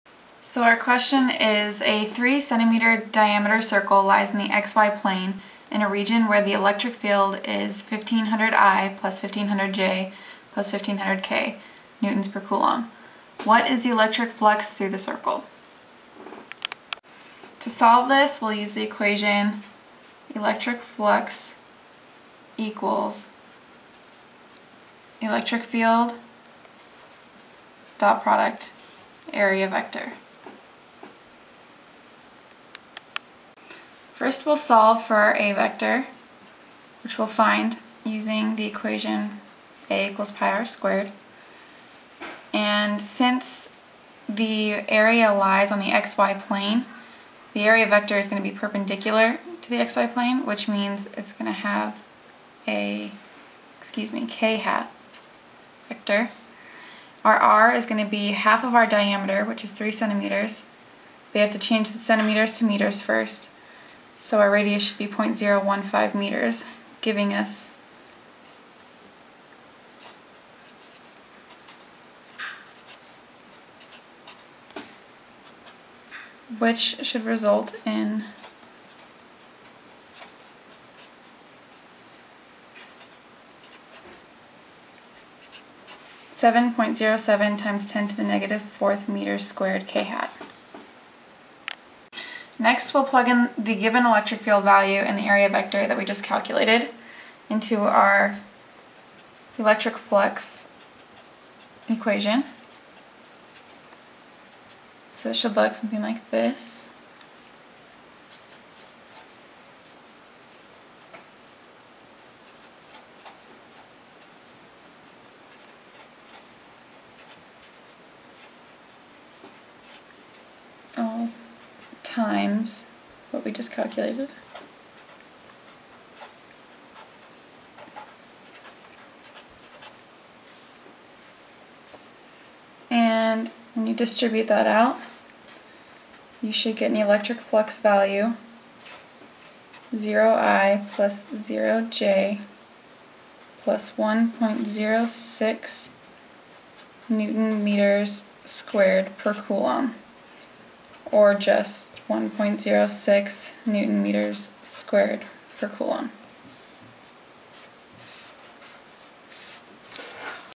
CH 28 #14
Physics for Scientists and Engineers Second Edition(Knight) Chapter 28, #14: A 3.0-cm-diameter circle lies in the xy-plane in a region where the electric field is E= (1500i+1500j+1500k)N/C. What is the electric flux through the circle?